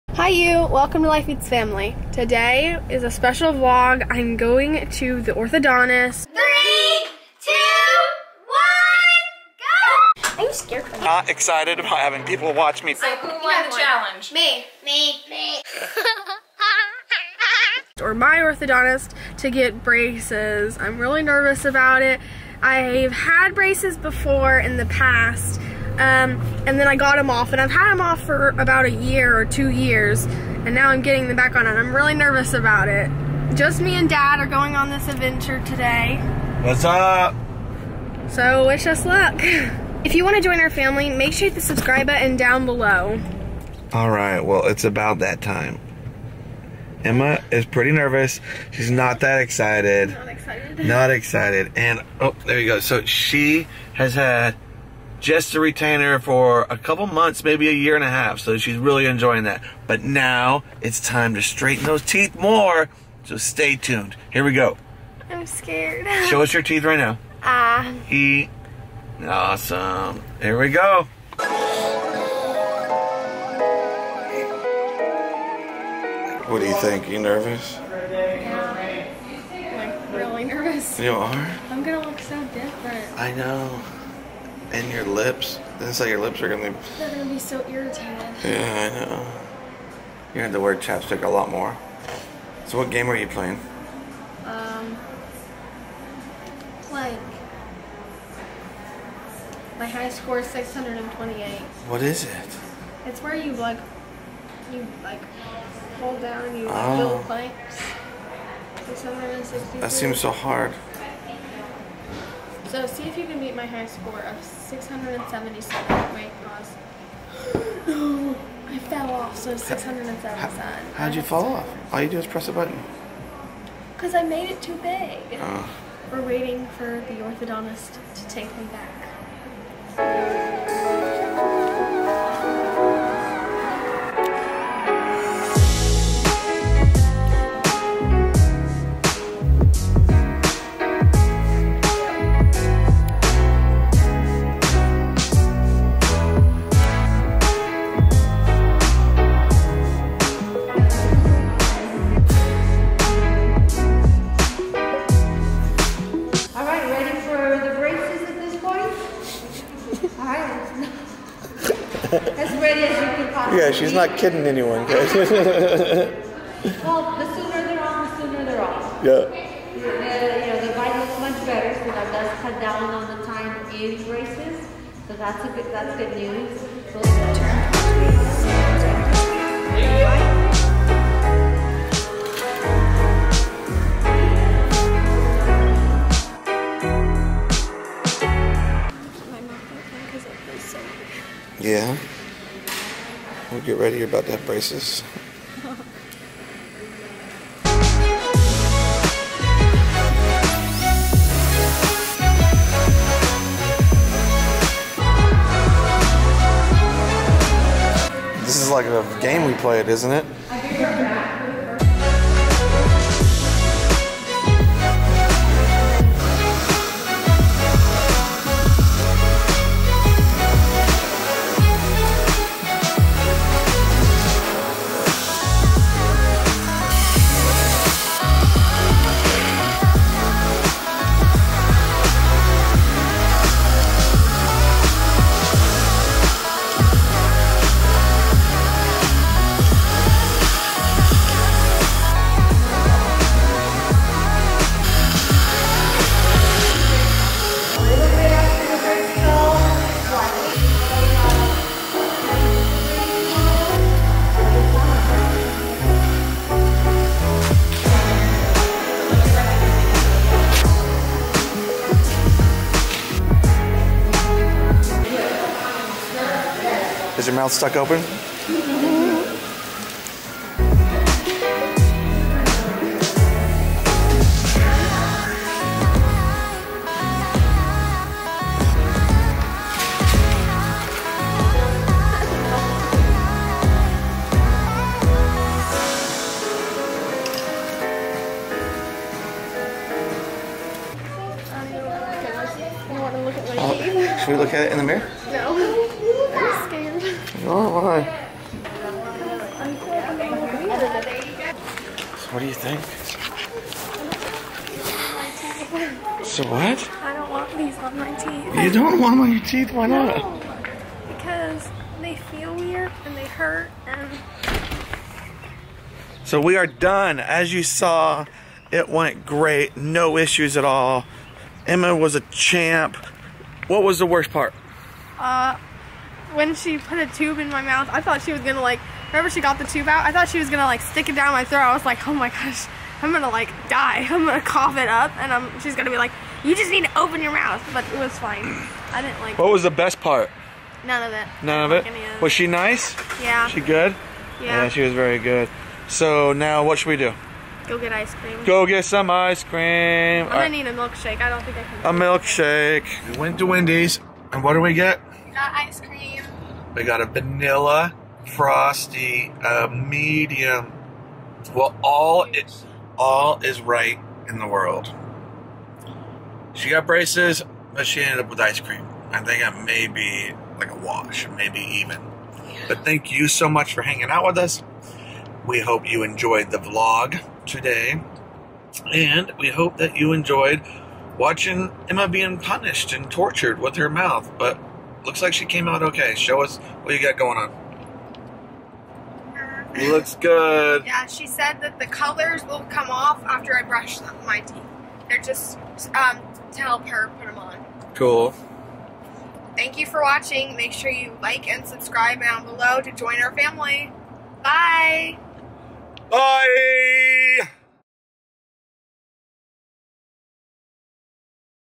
BRACES UPDATE | ORTHO VLOG
Special thanks to our amazing patron Rich. You rock!

Braces update from Emma.  Emma and Dan go to the orthodontist.  She gets her retainer removed and her second set of braces put on.  She is nervous and scared.  She doesn't want to look different and she doesn't want her mouth to hurt.  What is your favorite part of the vlog?

7433 Spout Springs Rd
Ste 101-1